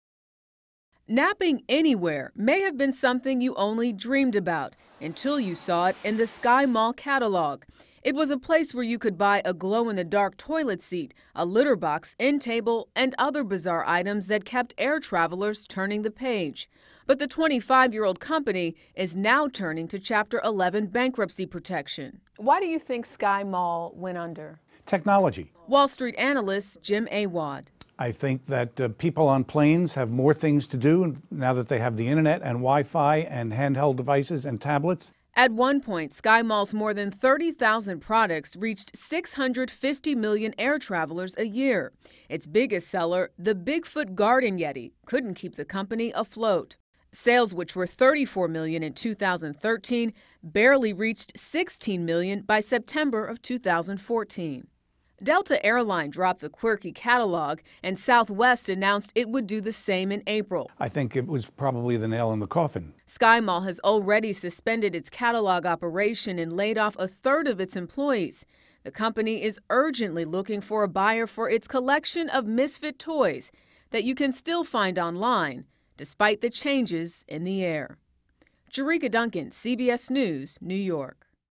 SkyMall falls victim to digital age
Known for its quirky offerings, SkyMall was the shopping destination for travelers 30,000 feet up in the air. But after 25 years of selling glow-in-the-dark toilet seats and mounted squirrel heads, the famed in-flight magazine filed for Chapter 11 bankruptcy protection. Jericka Duncan reports.